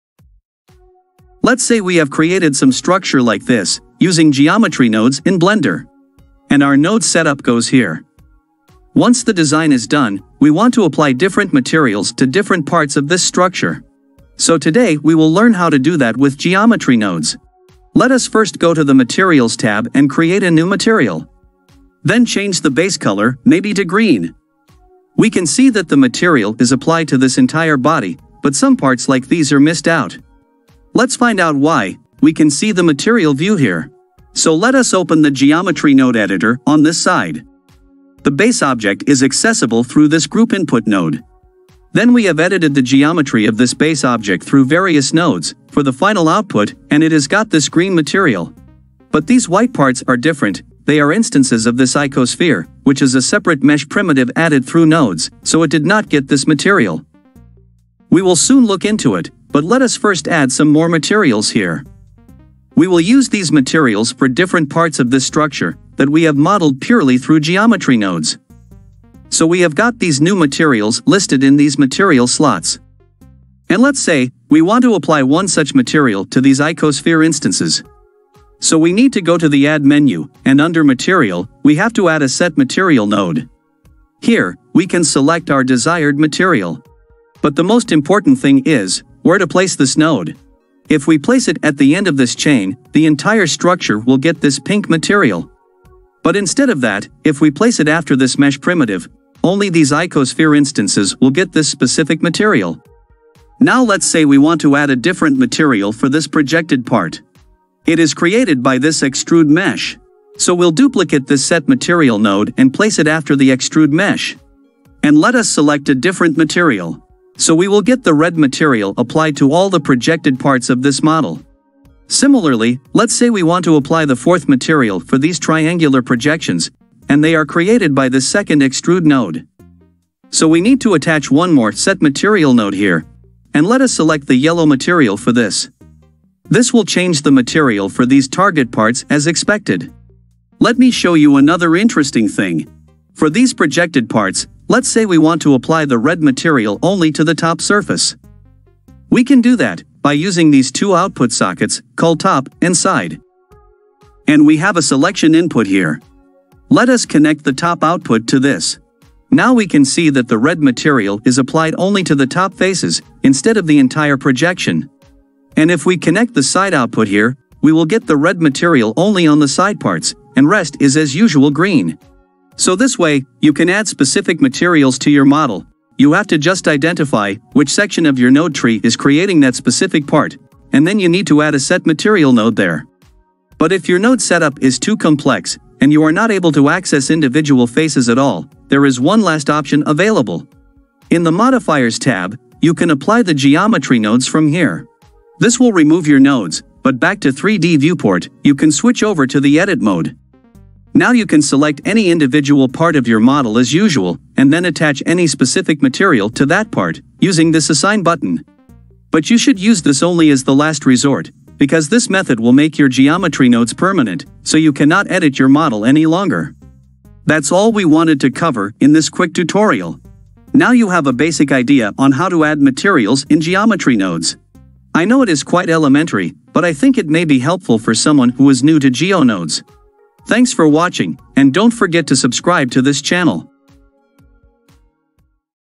How To Assign Materials In Geometry Nodes | Multiple Materials For Single Object | Set Material Node
In this tutorial, we have discussed how to assign any specific material to some object surface using geometry nodes in Blender. We use a Set Material node for this purpose. It is quite easy and basic, but it may be still helpful for the beginners in geometry nodes. Please watch the video for the complete tutorial.

Timestamp
00:00 How to use the Set Material node
02:20 How to use the Selection option
03:03 Apply geo nodes & assign any material